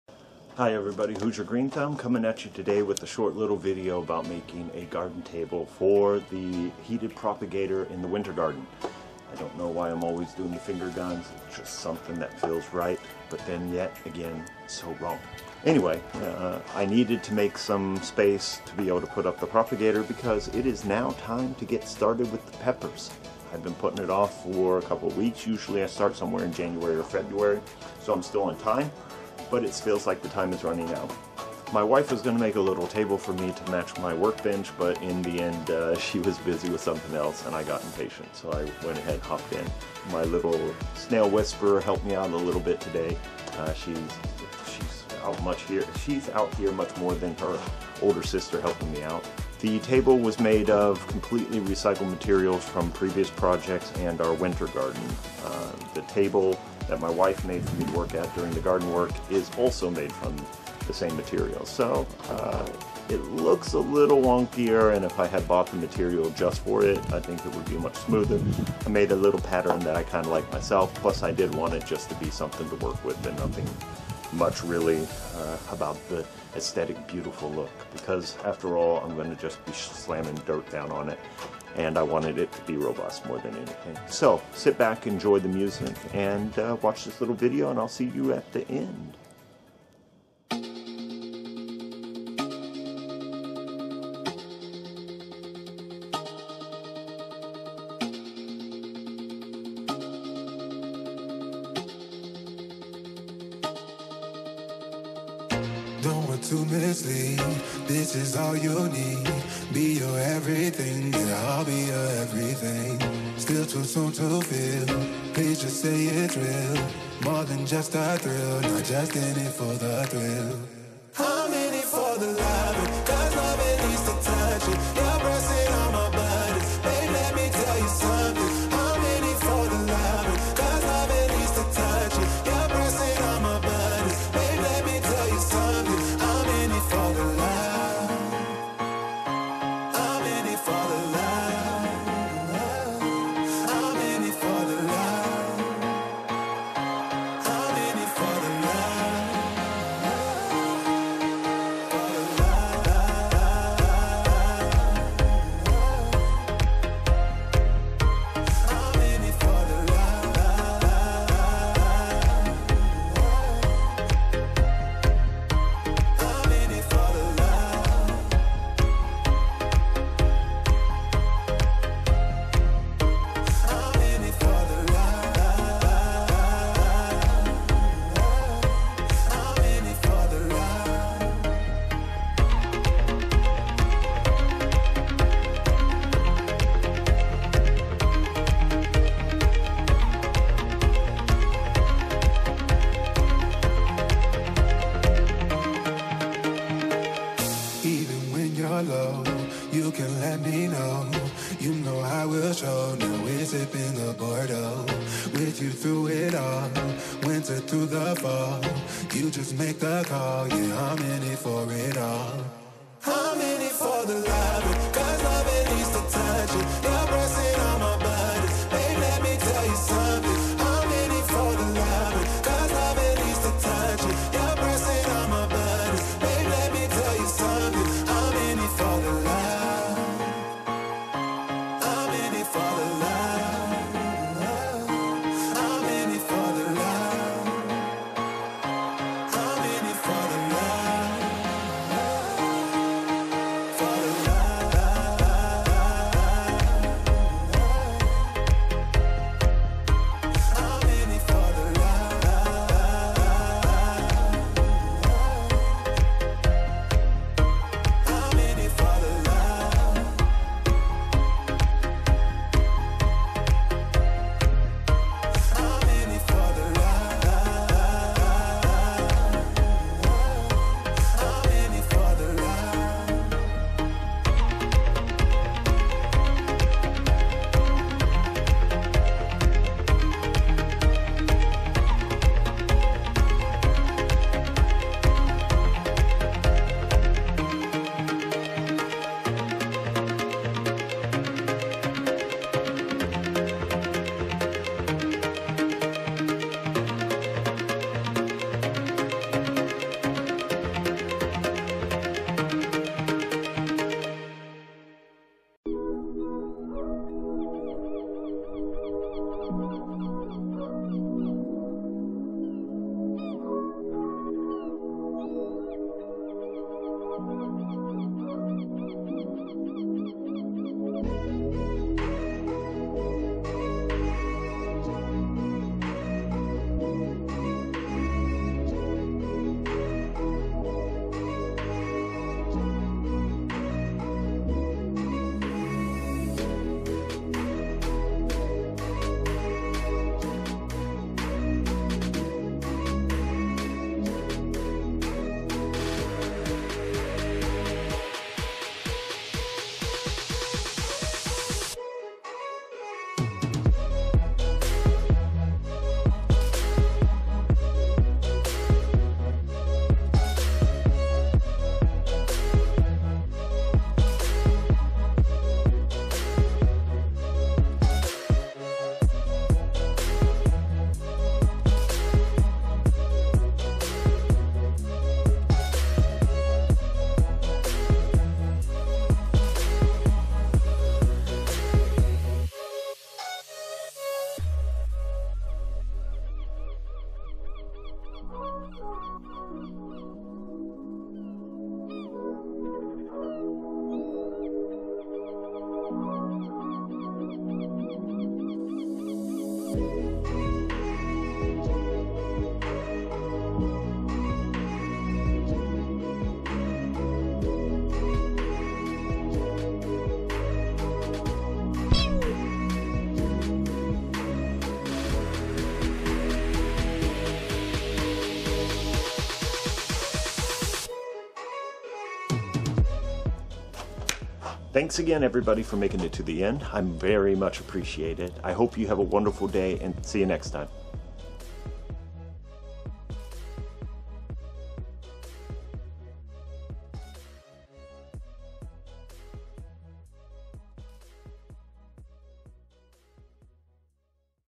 Building a Garden Table
Today I use more of the recycled wood from our winter garden to build a table to put my heated propagator and peppers plants to reside in our winter garden.  The snail whisperer is here to help out.

And...

And…